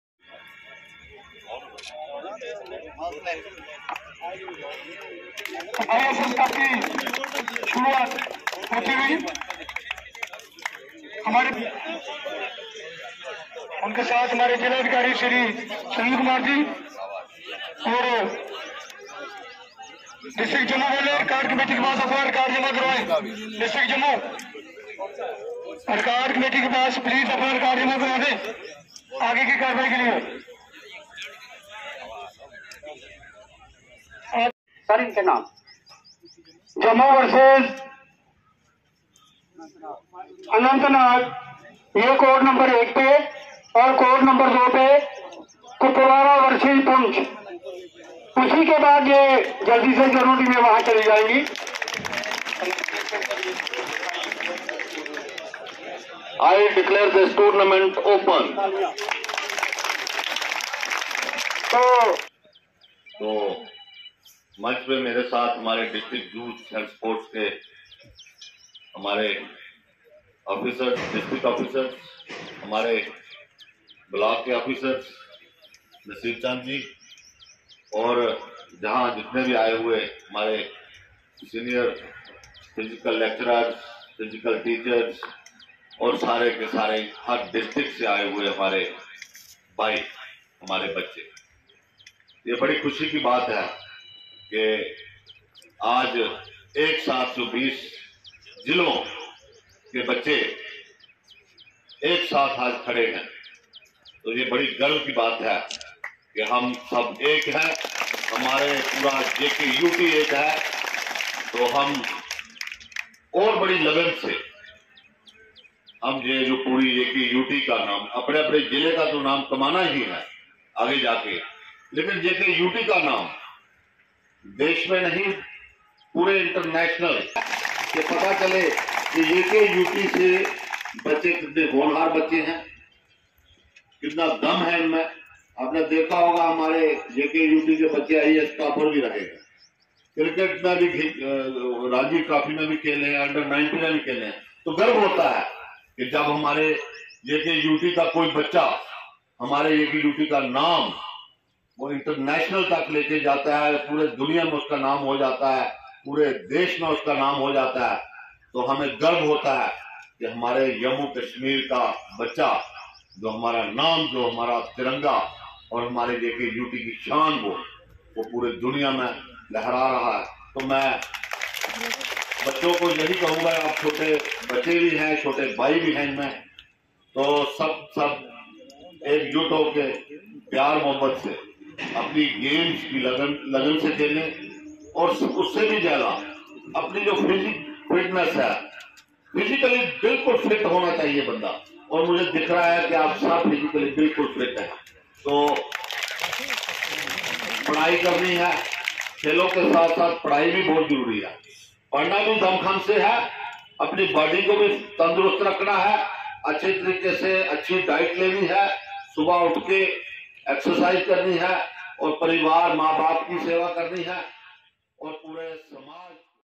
खेल विभाग द्वारा करवाई जा रही खेलों में आज कठुआ के डीडीसी के उपाध्यक्ष रघुनंदन सिंह बबलू मुख्य
खेल विभाग द्वारा करवाई जा रही खेलों में आज कठुआ के डीडीसी के उपाध्यक्ष रघुनंदन सिंह बबलू मुख्य अतिथि के रुप में पहुंचे